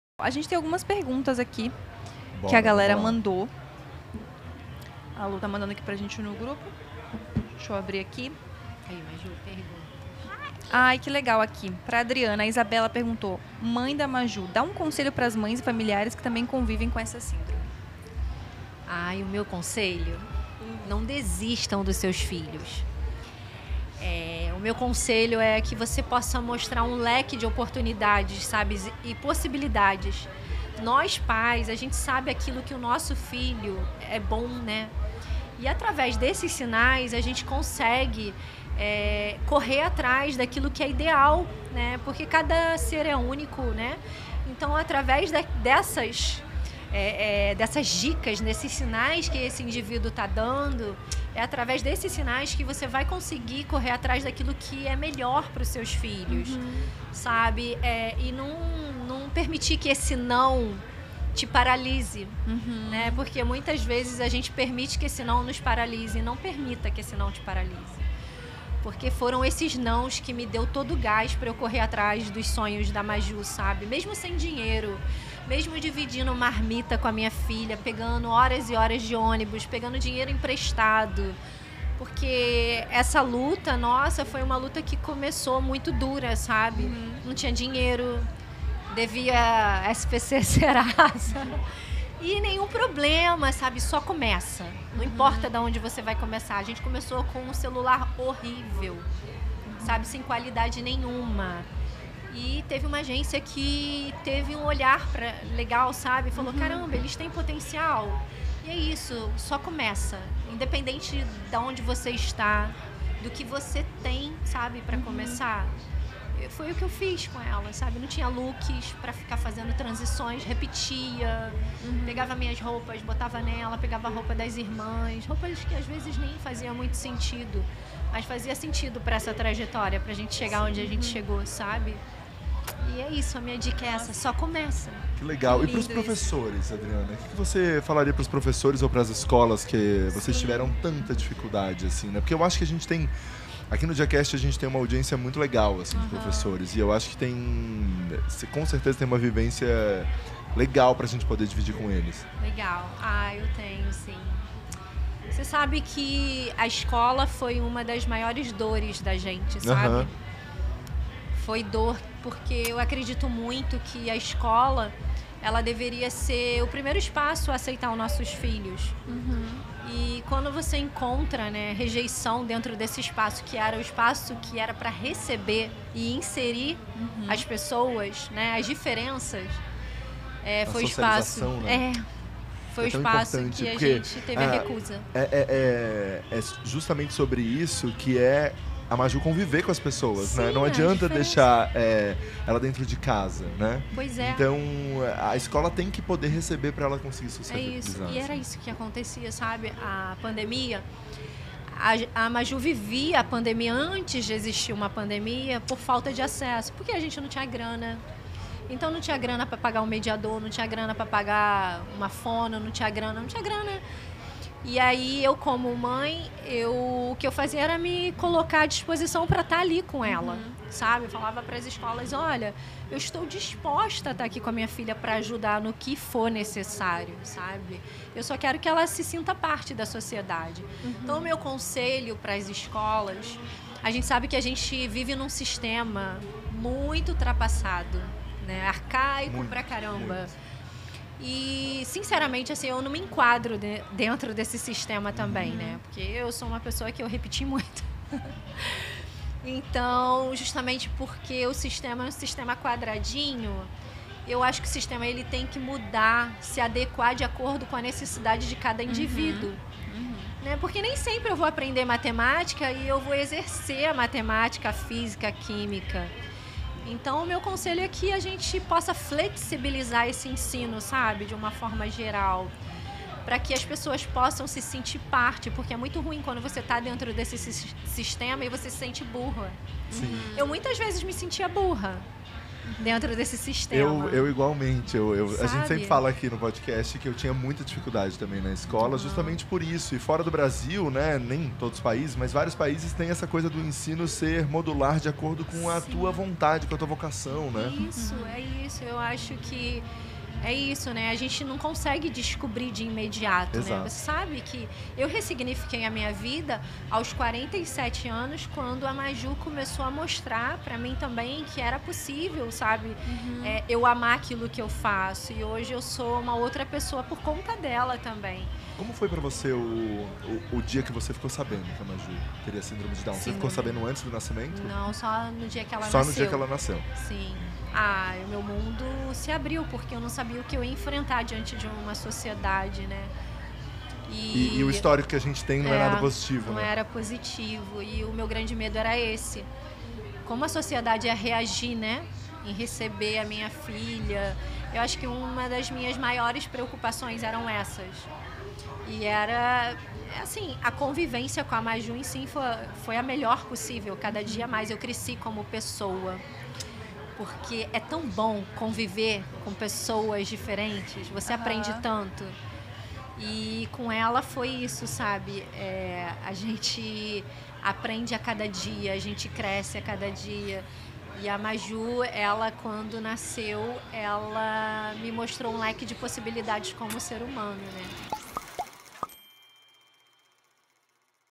Mãe de Maju de Araújo dá recado aos pais de pessoas com Síndrome de Down | Cortes DiaCast
Adriana, mãe de Maju de Araújo, responde às perguntas do chat e ressalta a importância das mães não desistirem de seus filhos, independente do que for. Bonito isso, né, gente? Já conheciam a história da Maju?

Redes da Maju de Araújo